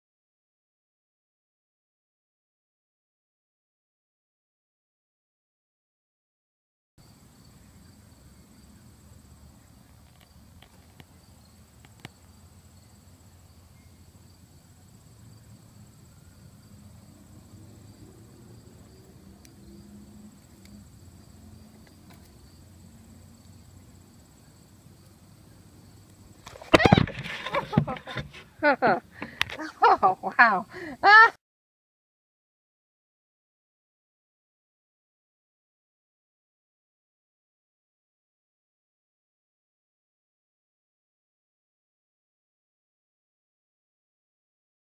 Sheri Ann Richerson from ExperimentalHomesteader.com Has A Garden Visitor
Sheri Ann Richerson from ExperimentalHomesteader.com saw this little critter sitting on her front ramp and wanted to see if she could get a video. What she didn't expect was for it to jump on her shirt and hang there. That was a shock!